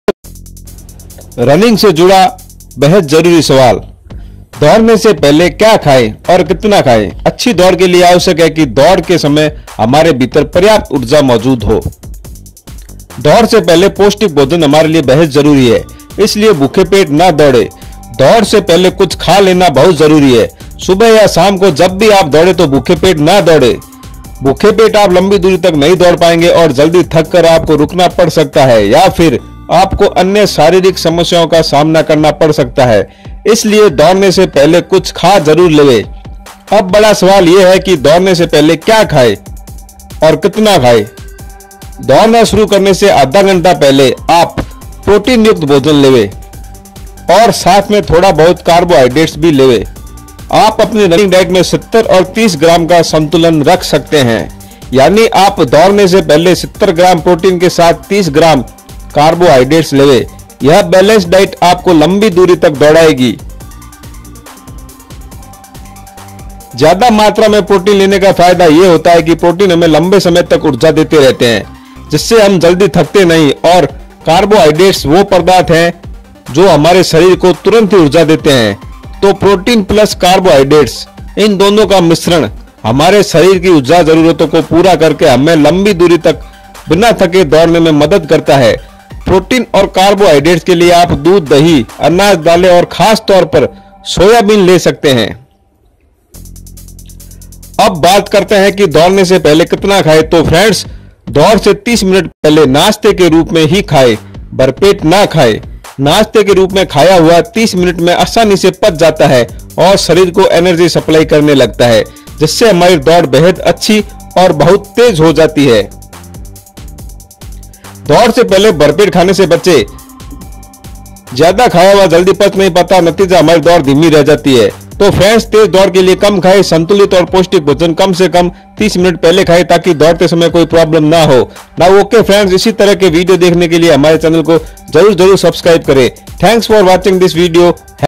दौड़ने से पहले क्या खाएं जिससे ताकत मिले ।। Running tips
Hello friends
This video is about what to eat before running ,